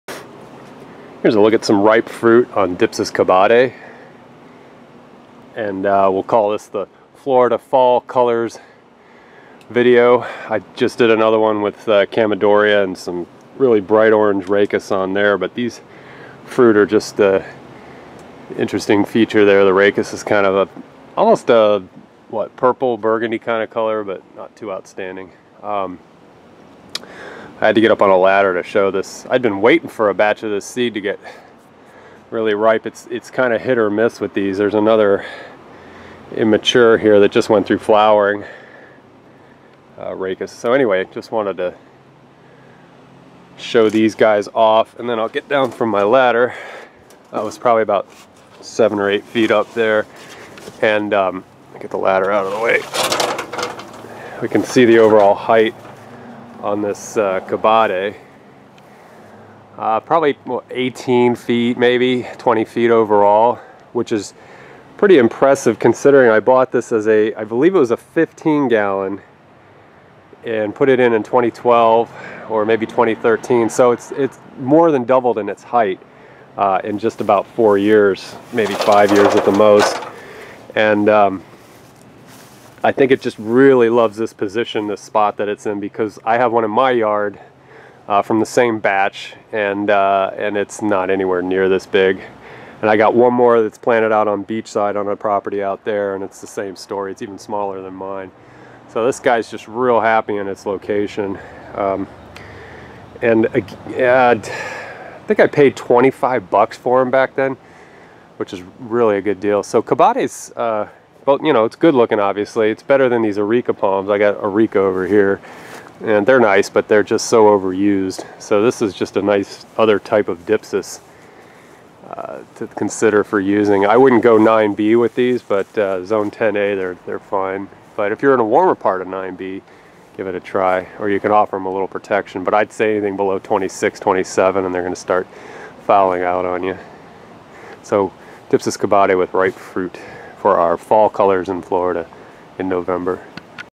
Dypsis cabadae Palm With Mature Fruit.
Here's a happy and vigorous Dypsis cabadae with lots of ripe fruit. I'm going to try to germinate the seeds and see what the success rate is.